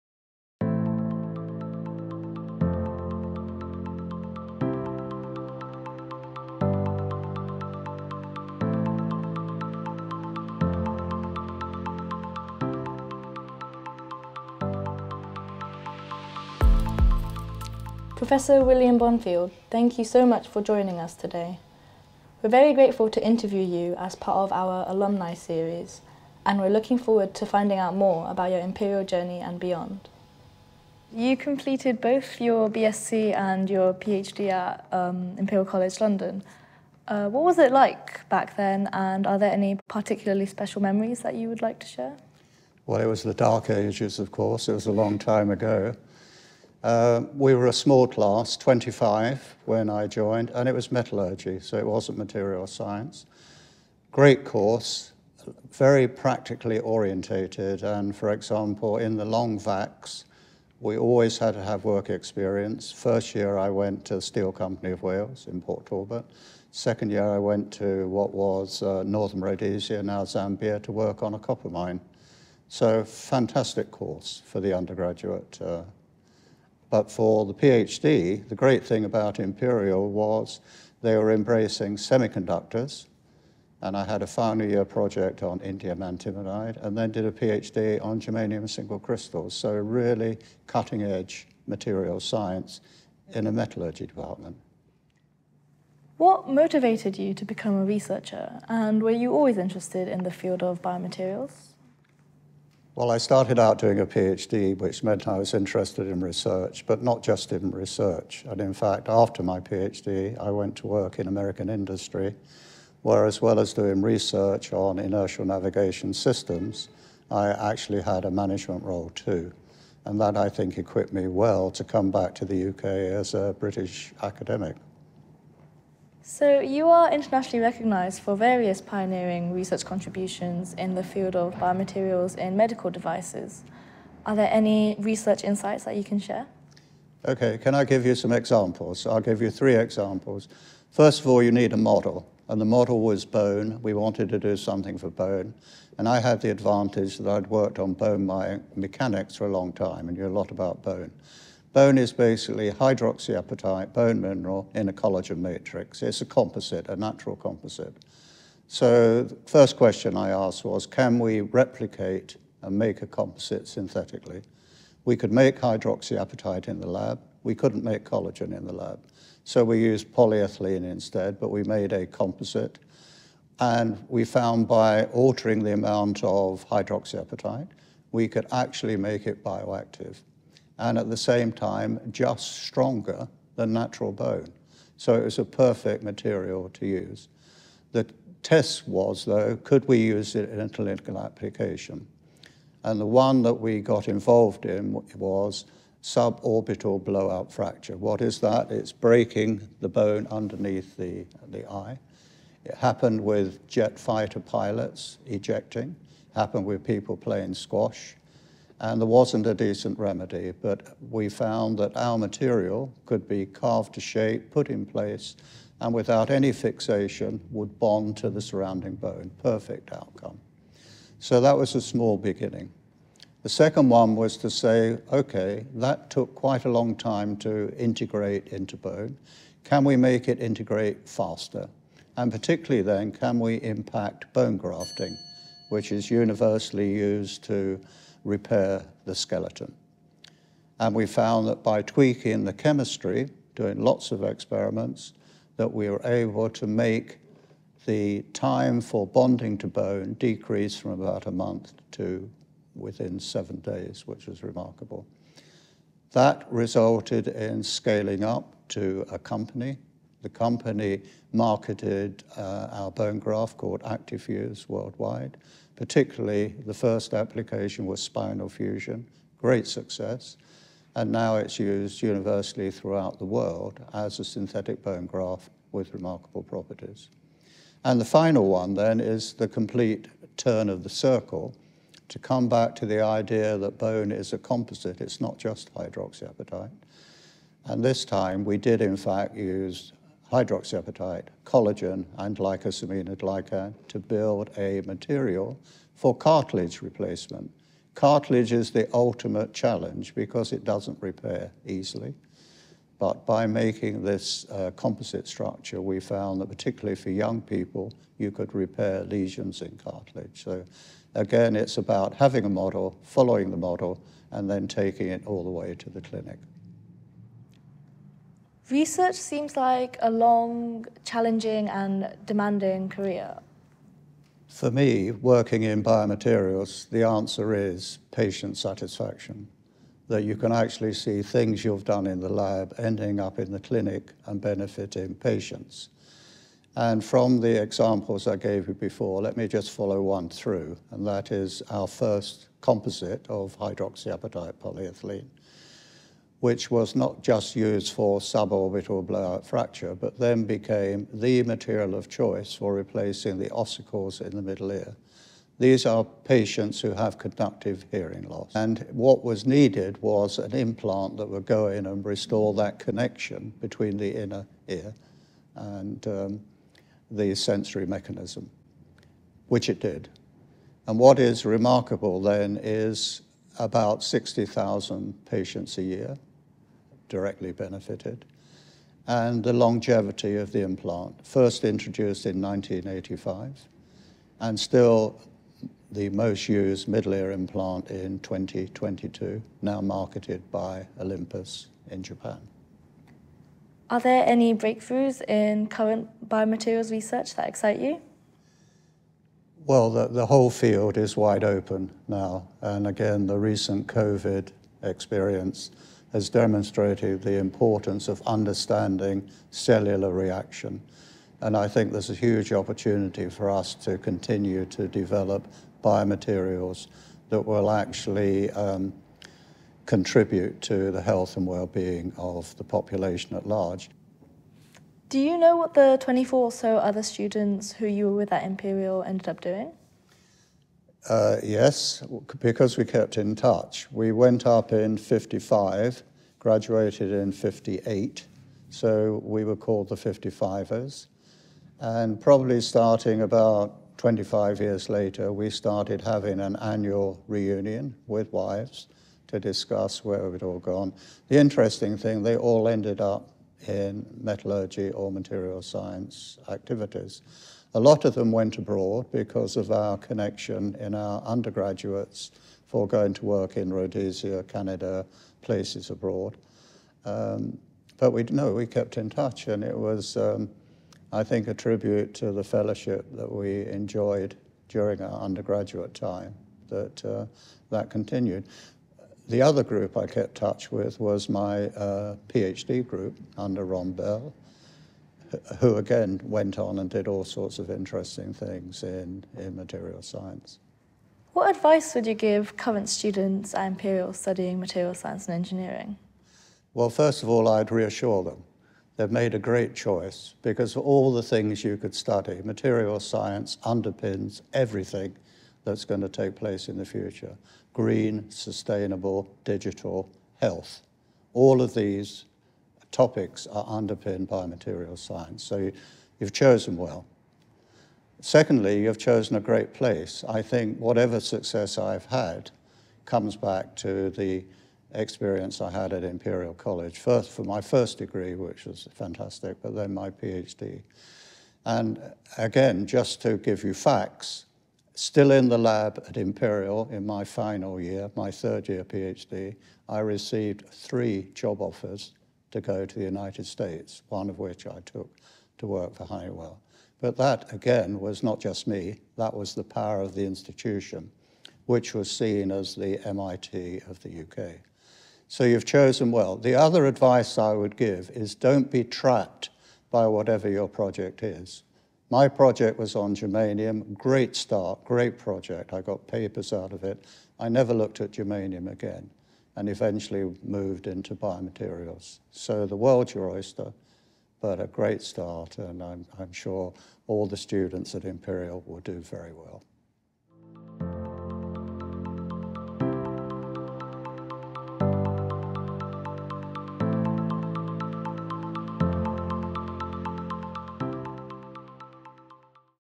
In conversation with Imperial Alumni: Professor William Bonfield CBE, FREng, FRS
Professor William Bonfield CBE, FREng, FRS, is an internationally recognised expert in biomaterials for medical devices. He graduated from ImperialCollegeLondon in 1955 and has since made pioneering research contributions. He has won many notable awards for his work.

Our undergraduate student Kalea Booth had the opportunity to ask her questions to Professor Bonfield for an insightful interview 📽️

Hear about his journey from his time at ImperialCollegeLondon to his international recognition for his research. Discover what inspired his passion for MaterialsScience and Engineering and the highlights of his accomplished career.